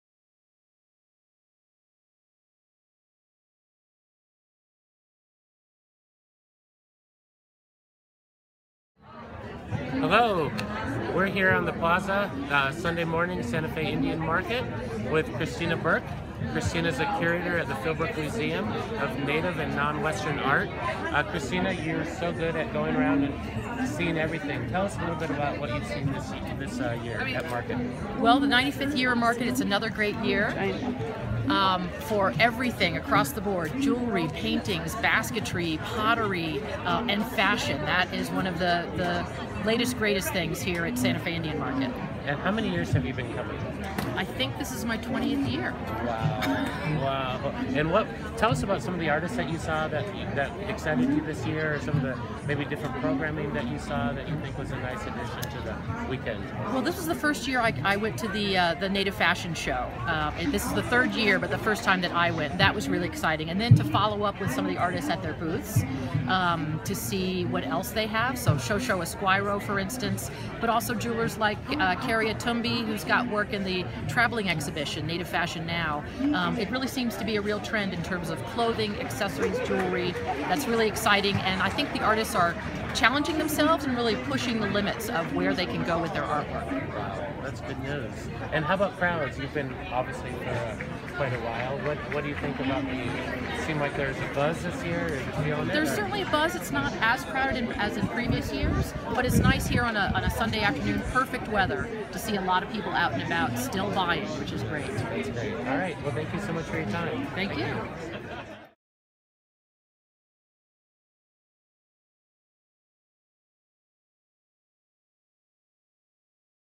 Market Chat with curator Christina Burke — Indian Market 2016
A brief chat with curator Christina Burke of the Philbrook Museum of Art in Tulsa, Oklahama, at Indian Market 2016.